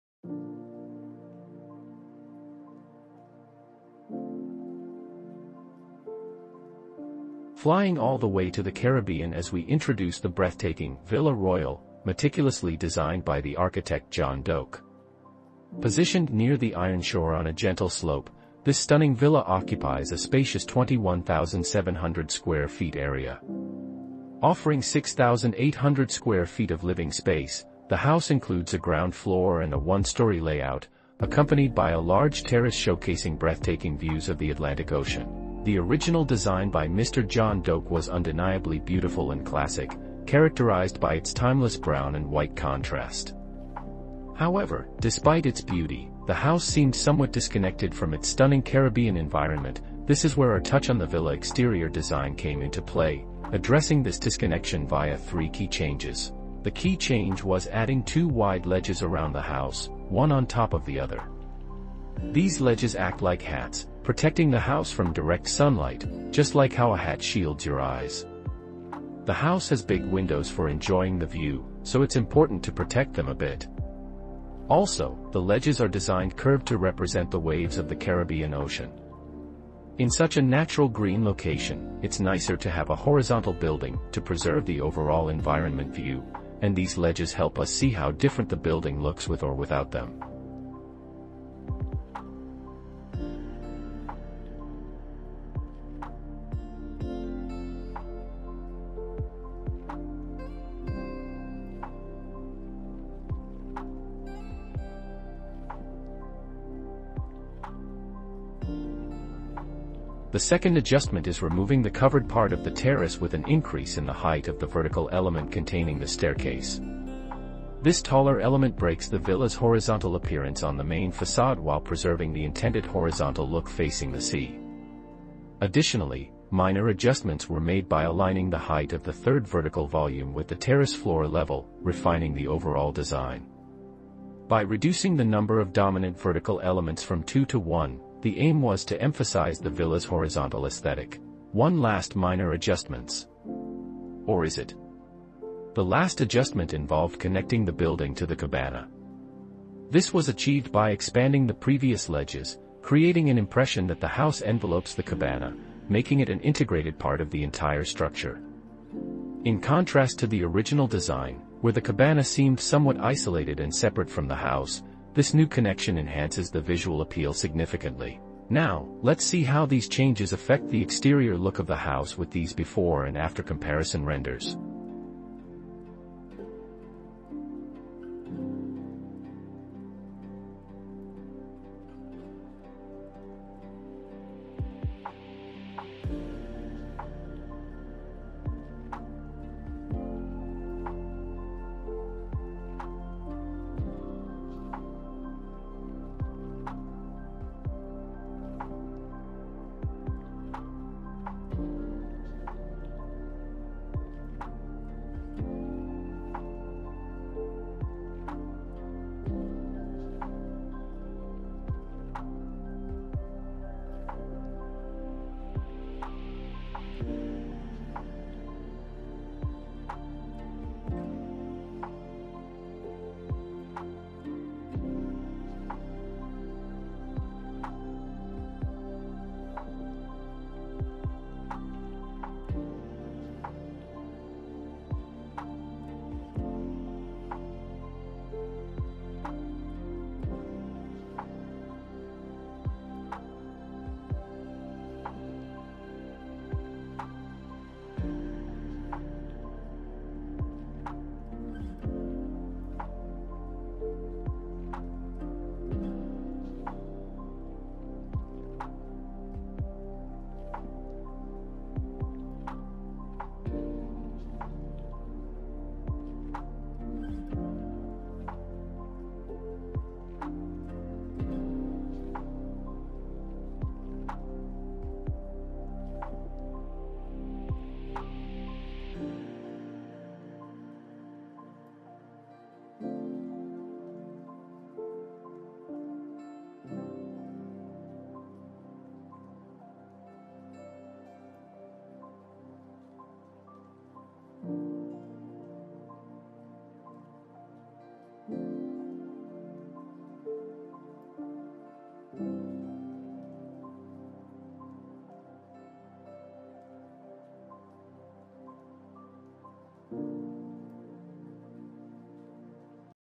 Caribbean Mirage: A Modern Twist on Classic Villa Design . Twinmotion
Discover the mesmerizing transformation of a classic villa designed by Mr. John Doak into a stunning masterpiece that seamlessly blends with the breathtaking Caribbean landscape. Witness the meticulous adjustments and three key changes implemented to bridge the gap between elegance and environmental harmony. From the addition of protective ledges mimicking ocean waves to terrace modifications and an integrated design approach, explore how this villa evolved into a sanctuary that celebrates architectural beauty while embracing its natural surroundings.

00:00 Introduction to the original design.
00:56 The first key change.
01:30 Side by side.
01:50 The second key change.
02:22 The last minor adjustment.
02:49 Renders before and after.